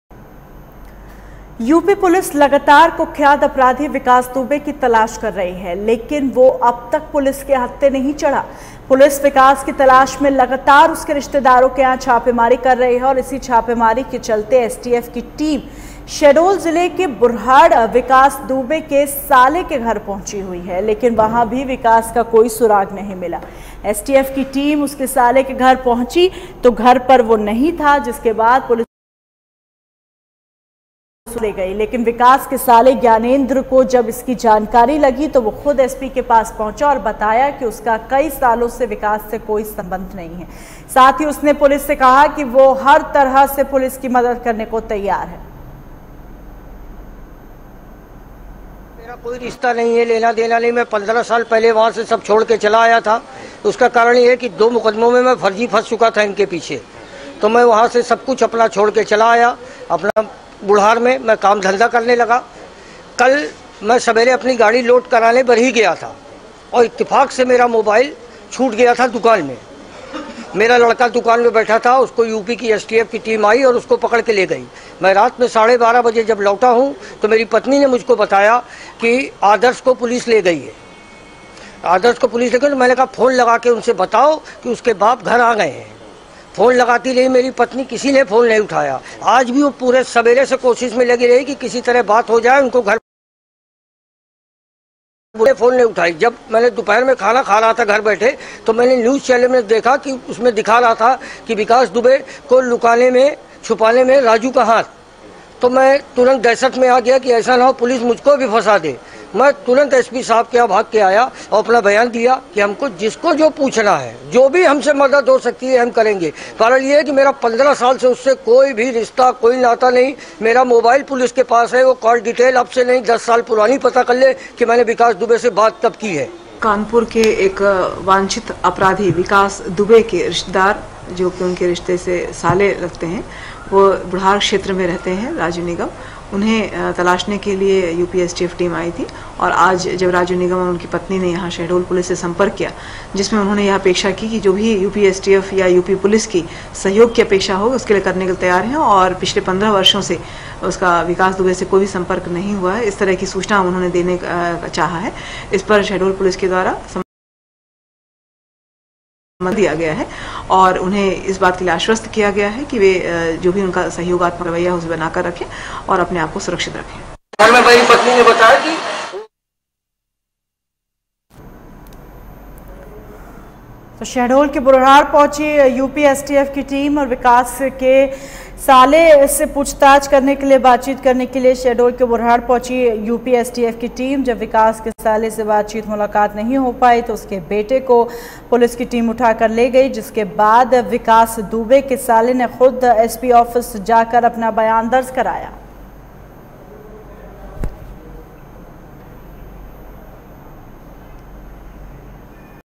शहडोल विकास दुबे के साले के घर एसटीएफ़ की छापेमारी
हमसे जुड़े रहने के लिए हमें फॉलो करें